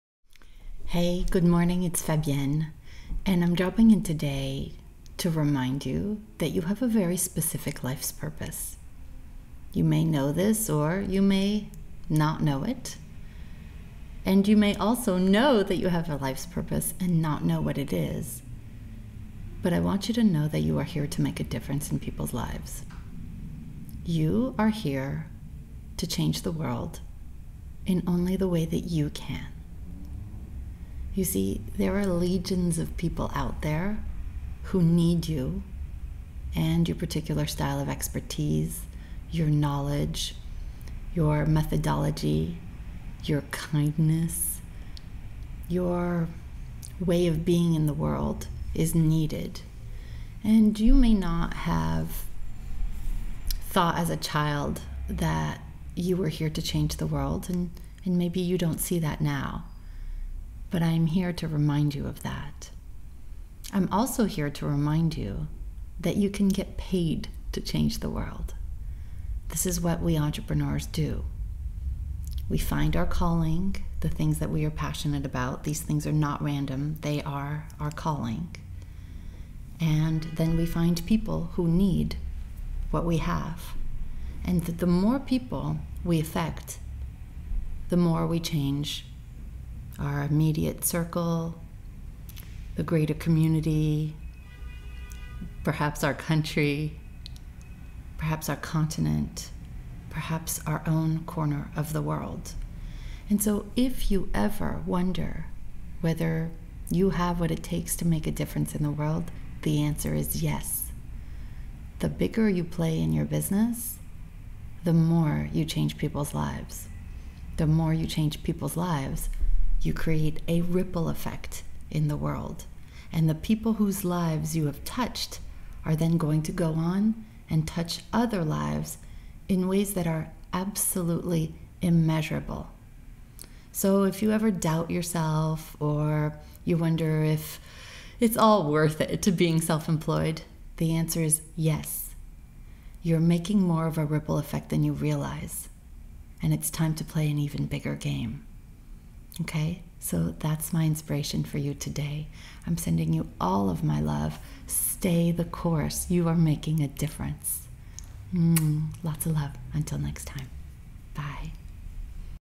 You can change the world (and get paid for it)
| Today I have a new video to remind you that you have a very specific life's purpose.

You may know this, or you may not know it. You may also know that you have a life's purpose, and not know what it is.

But I want you to know that you’re here to make a difference in people's lives. You’re here to change the world in only the way that you can.

Here’s my inspiration message for you today.